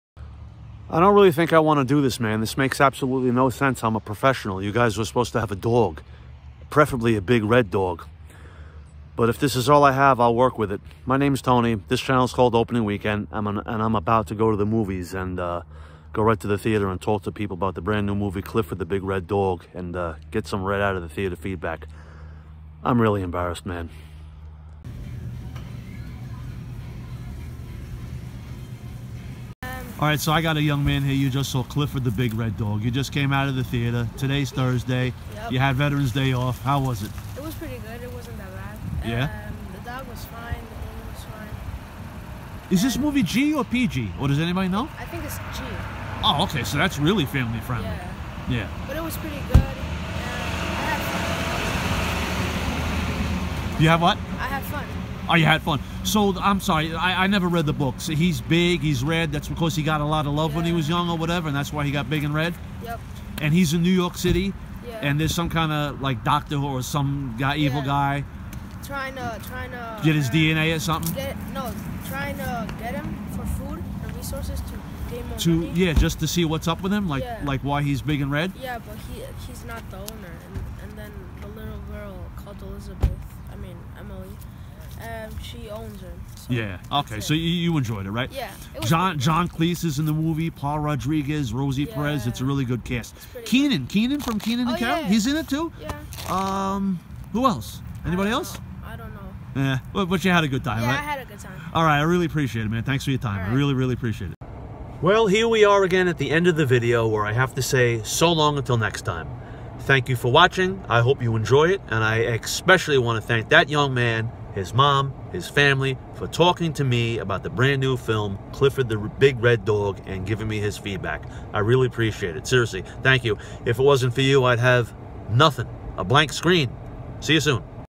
‘Clifford The Big Red Dog’ Movie Review/Out Of Theater Reaction
A nice young man gives his review